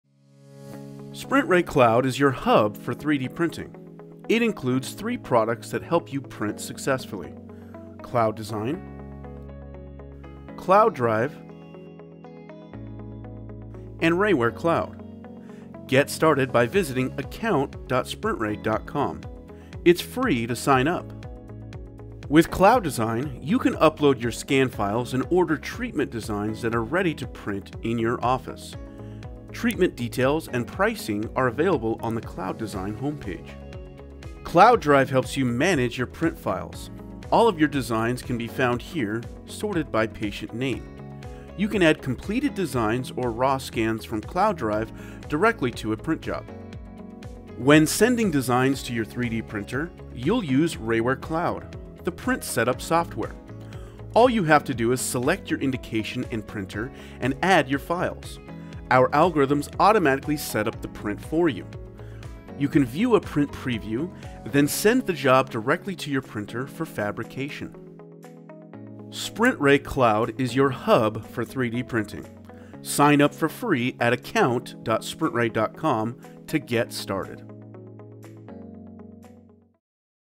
Getting Started - SprintRay Cloud
Welcome to the world of dental 3D printing with SprintRay Cloud! This powerful platform is your gateway to setting up print jobs, ordering patient treatment designs, and tracking your progress. Start here to learn the basics.